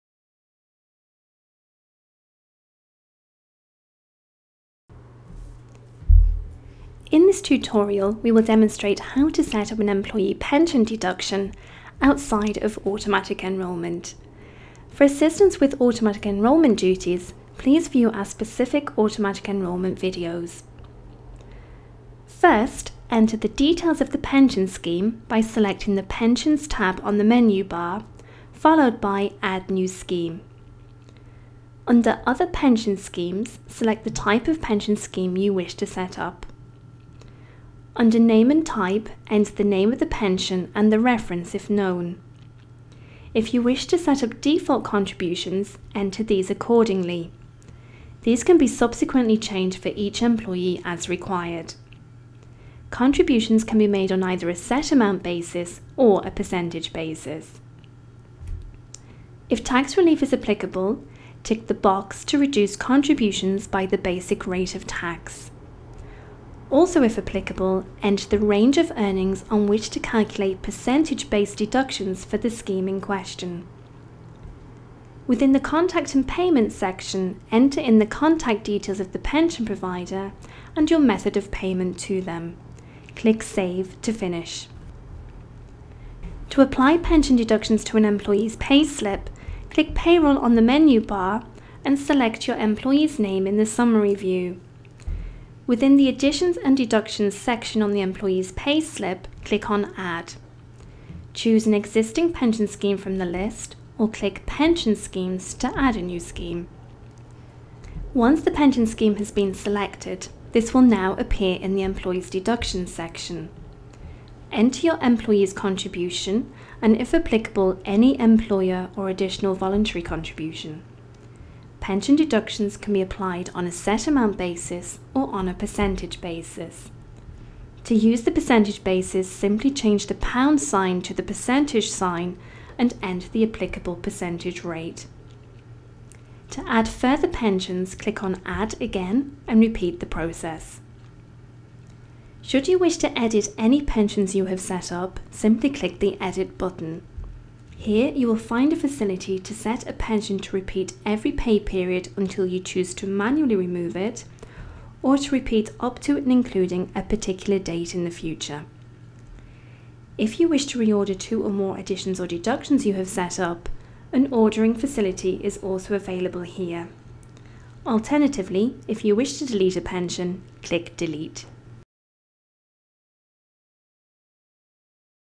Pension Deductions
In this tutorial we will demonstrate how to set up an employee pension deduction, outside of Automatic Enrolment. For assistance with Automatic Enrolment duties, please view our specific automatic enrolment videos.

First, enter the details of the pension scheme by selecting the ‘Pensions’ tab on the menu bar, followed by ‘Add New Scheme’.

Under ‘Other Pension Schemes’ select the type of pension scheme you wish to set up.

Under Name and Type, enter the name of the pension and the reference, if known.

If you wish to set up default contributions, enter these accordingly. These can be subsequently changed for each employee as required. Contributions can be made on either a set amount basis or a percentage basis.

If tax relief is applicable, tick the box to reduce contributions by the basic rate of tax.

Also, if applicable, enter the range of earnings on which to calculate %-based deductions for the scheme in question.

To apply pension deductions to an employee’s payslip,  click ‘Payroll’ on the menu bar and select your employee’s name in the summary view.

Within the ‘Additions & Deductions’ section on the employee’s payslip, click on ‘Add’. Choose an existing pension scheme from the list or click ‘Pension Schemes’ to add a new scheme.

Once the pension scheme has been selected, this will now appear in the employee’s deductions section.

Enter your employee’s contribution and if applicable, any employer or additional voluntary contribution.

Pension deductions can be applied on a set amount basis or on a percentage basis. To use the percentage basis, simply change the £ sign to the % sign and enter the applicable percentage rate.

To add further pensions, click on ‘Add’ again and repeat the process.

Should you wish to edit any pensions you have set up, simply click the ‘Edit’ button. Here you will find a facility to set a pension to repeat every pay period until you choose to manually remove it or to repeat up to and including a particular date in the future.

If you wish to re-order two or more additions or deductions you have set up, an ‘Ordering’ facility is also available here.

Alternatively, if you wish to delete a pension, click ‘Delete’.